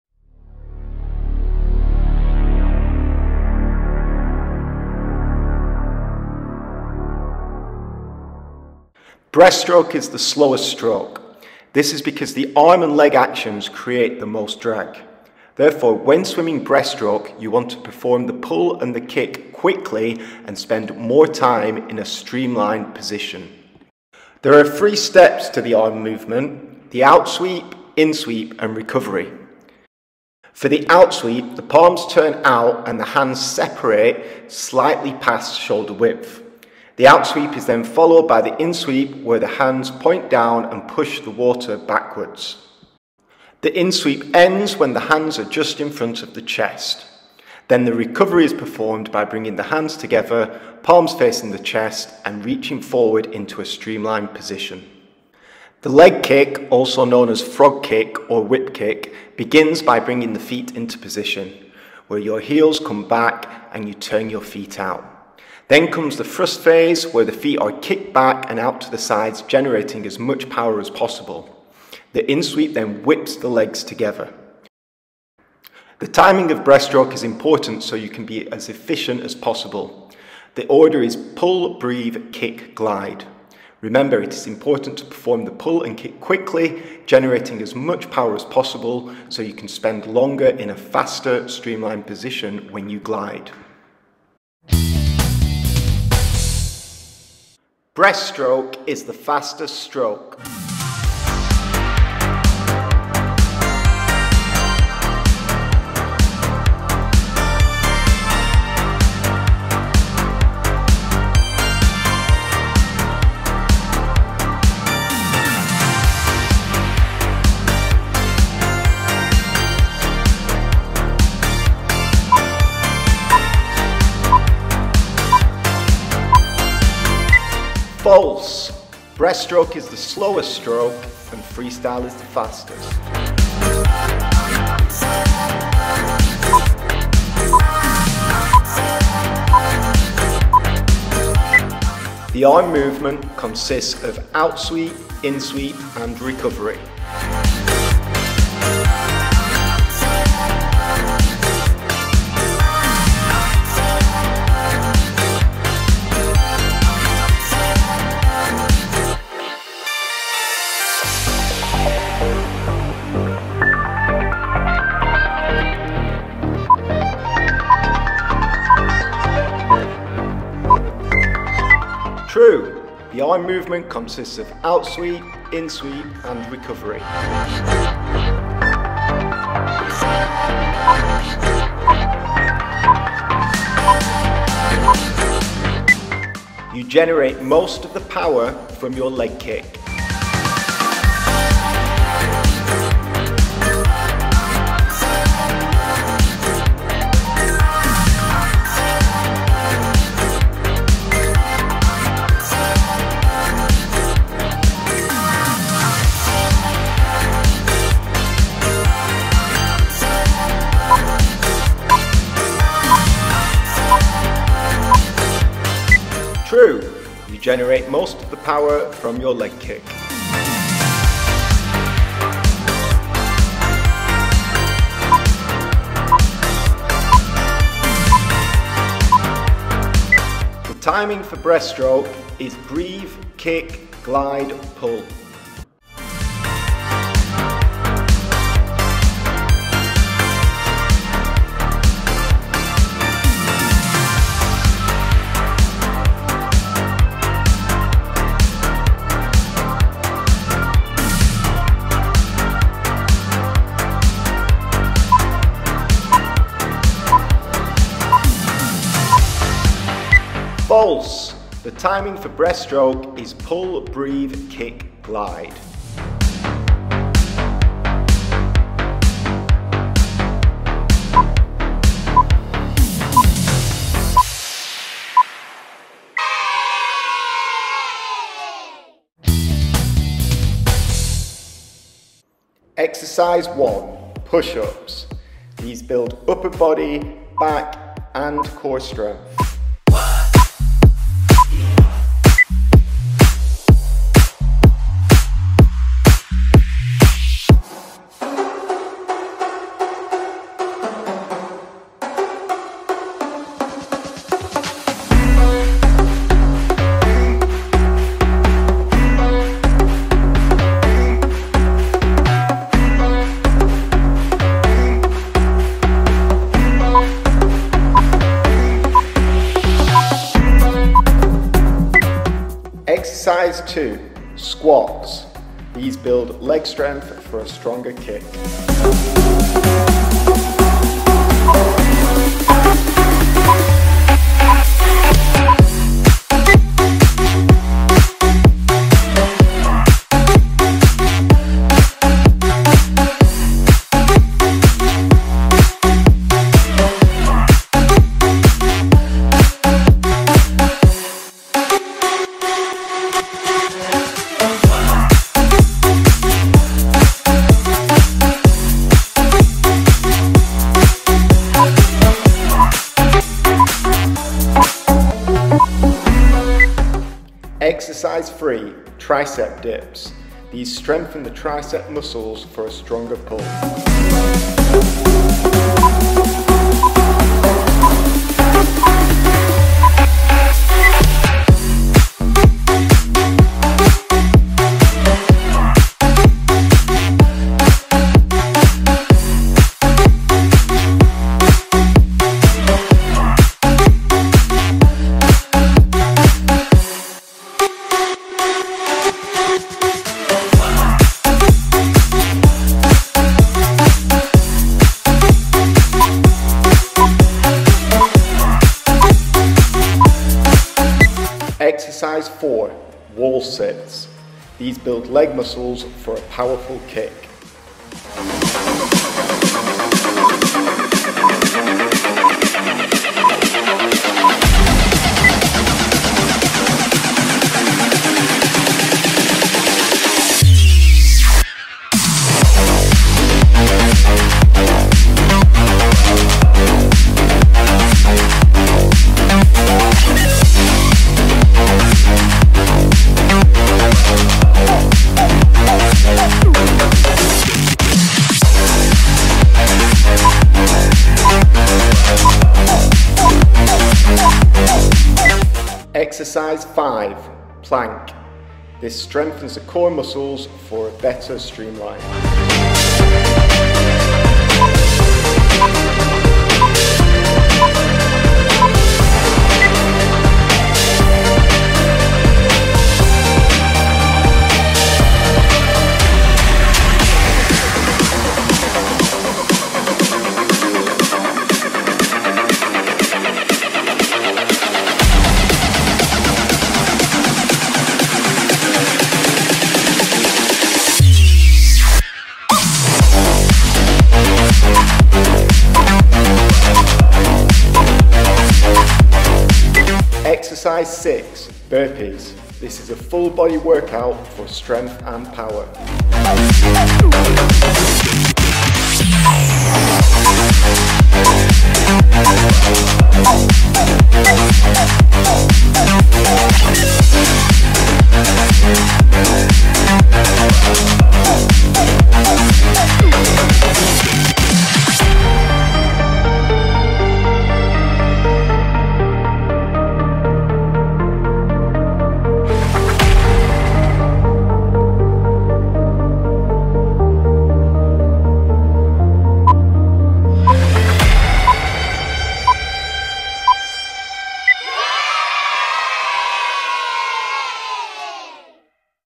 How to Swim BREASTSTROKE - Virtual Swimming PE Lesson
Learn the correct technique for swimming Breaststroke.

Theory on the importance of streamline, correct arm movement, leg kick and timing.

Warm up ' land training activity.

Finish with a Breaststroke home workout to improve strength and power.